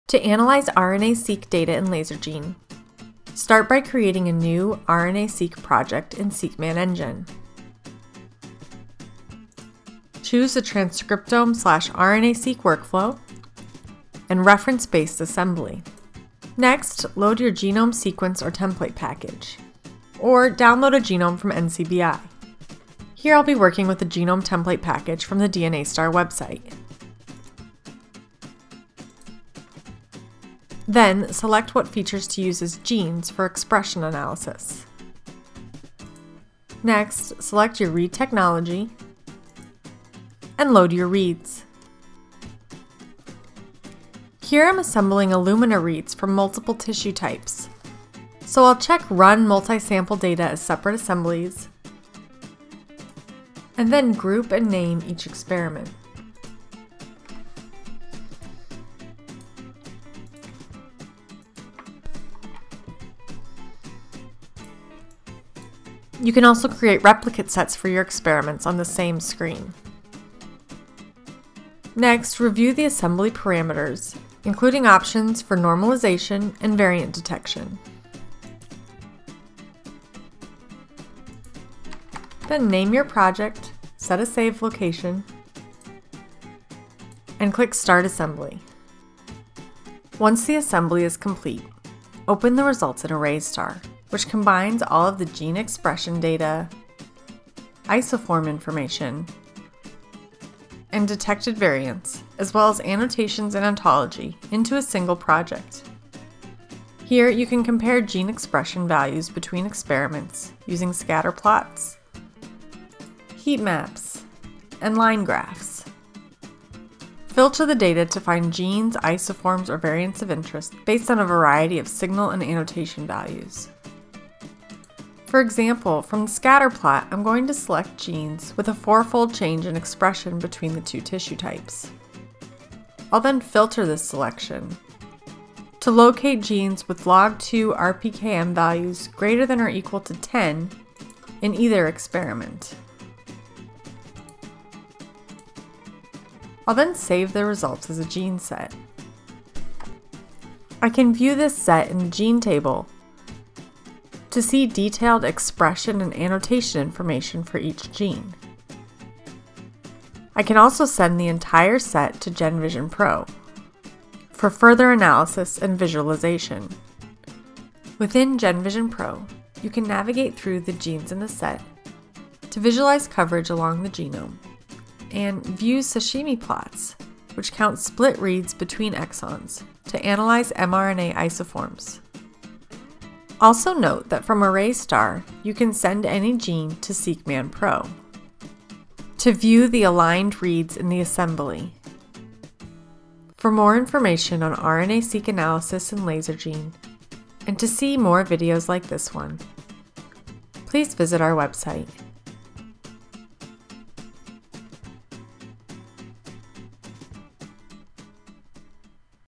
DNASTAR - RNA-Seq Analysis in Lasergene Genomics Suite
See how to align and analyze RNA-Seq data in Lasergene Genomics Suite. Use interactive views to visualize expression data, create gene sets, and asses mRNA isoforms.